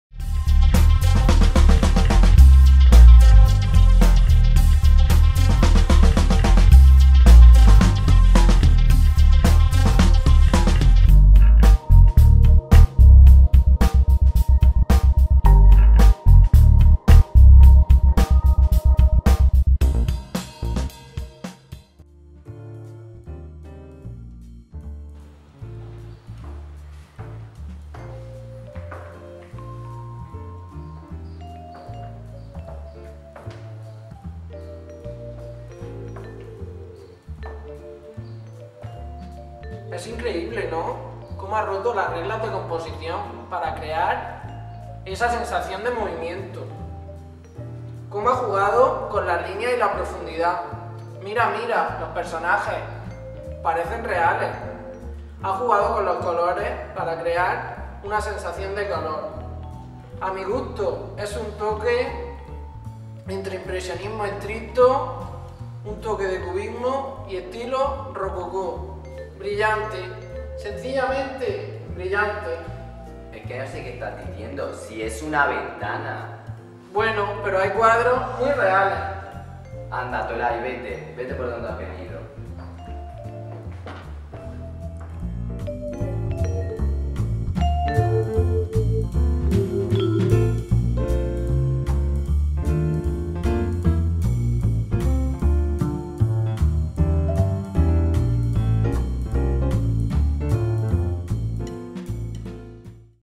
zekiGraphic - sketch comico "EL MUSEO"
sketch cómico titulado "EL MUSEO" donde sucede una cosa algo curiosa.